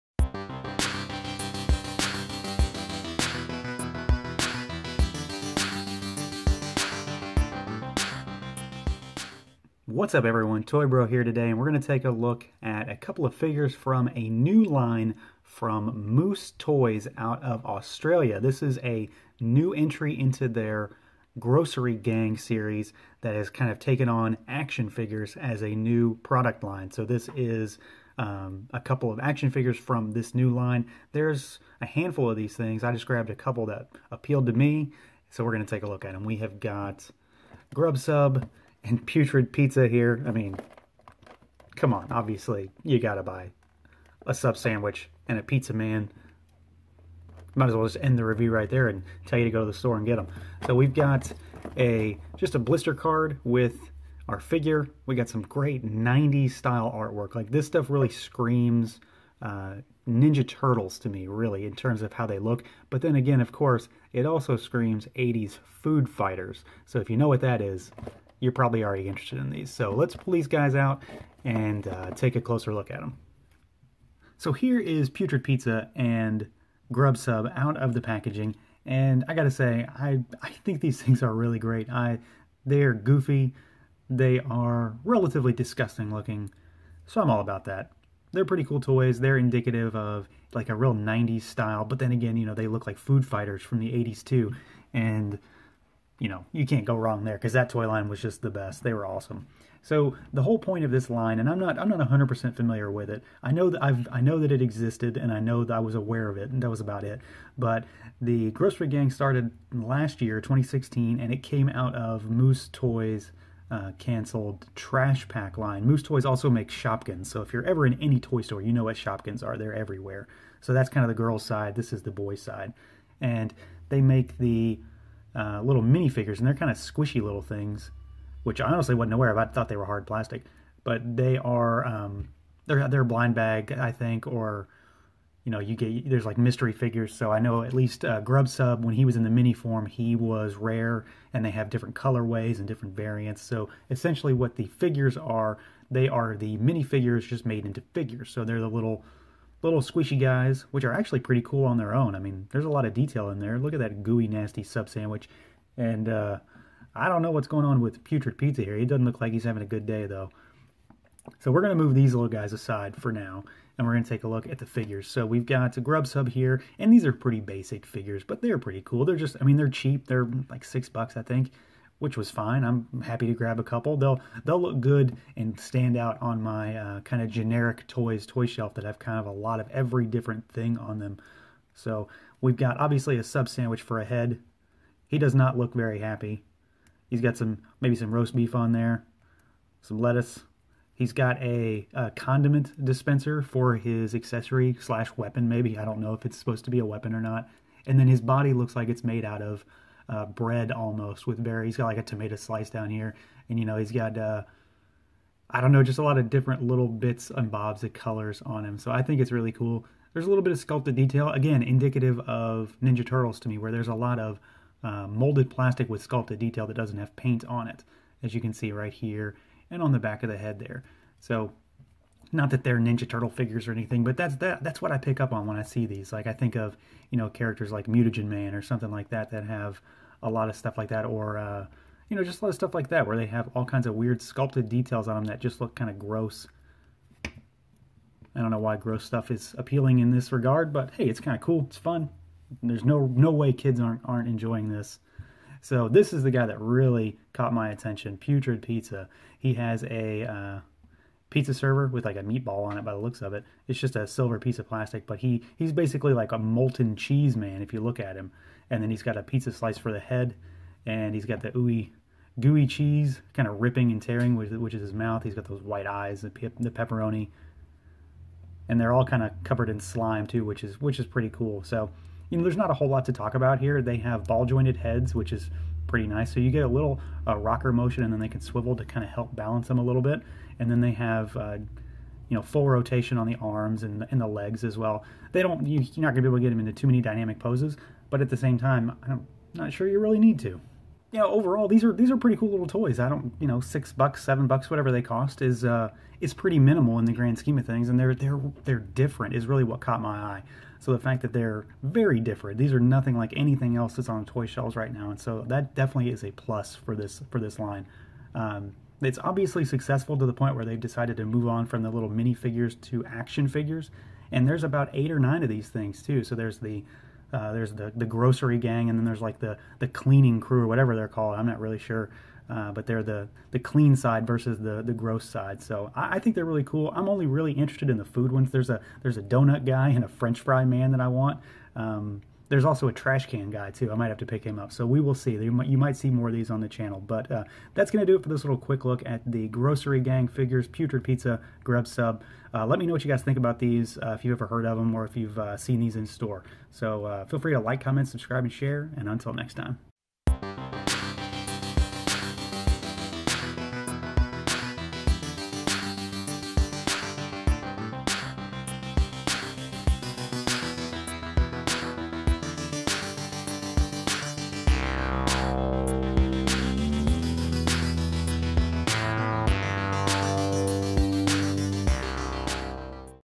The Grossery Gang PUTRID PIZZA & GRUB SUB Action Figure Toy Review
Moose Toys' new Grossery Gang Action Figures!

Follow Toy Bro on social media and never miss a video!